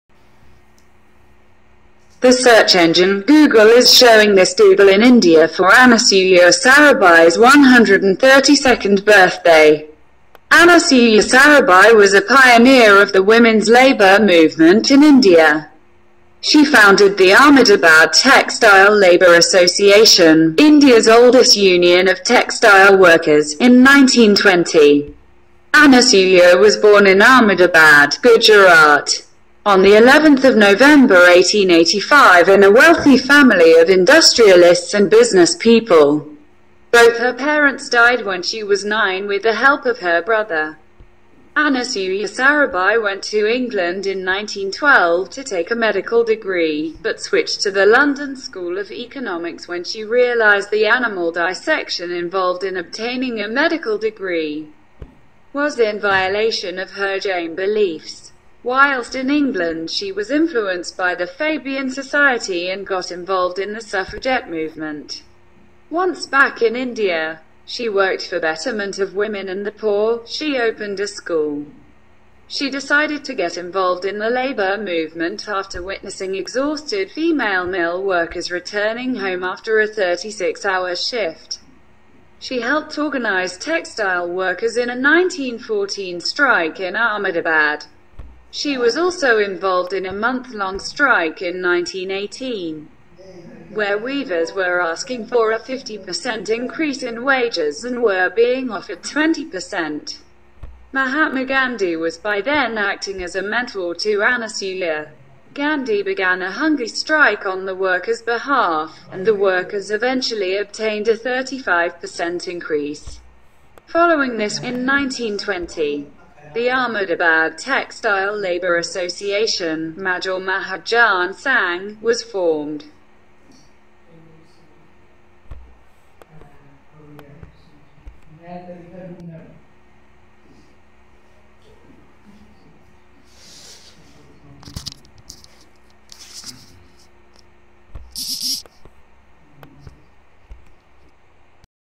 Who was Anasuya Sarabhai - Google Doodle
The Search Engine Google is showing this Doodle in India for Anasuya Sarabhai’s 132nd Birthday.

Anasuya Sarabhai was a pioneer of the women’s labour movement in India. She founded the Ahmedabad Textile Labour Association , India's oldest union of textile workers, in 1920.

Anasuya was born in Ahmedabad, Gujarat,  on 11 November 1885 in  a wealthy family of industrialists and business people.  Both her parents died when she was nine

 With the help of her brother, Anasuya Sarabhai   went to England in 1912 to take a medical degree, but switched to the London School of Economics when she realised the animal dissection involved in obtaining a medical degree, was in violation of her Jain beliefs. Whilst in England she was influenced by the Fabian Society and got involved in the Suffragette movement.

Once back in India, she worked for betterment of women and the poor; she opened a school. She decided to get involved in the labour movement after witnessing exhausted female mill workers returning home after a 36-hour shift. She helped organise textile workers in a 1914 strike in Ahmedabad. She was also involved in a month-long strike in 1918, where weavers were asking for a 50 per cent increase in wages and were being offered 20 per cent. Mahatma Gandhi  was by then acting as a mentor to Anasuya. Gandhi began a hunger strike on the workers' behalf, and the workers eventually obtained a 35 per cent increase. Following this, in 1920, the Ahmedabad Textile Labour Association (Majoor Mahajan Sangh)  was formed.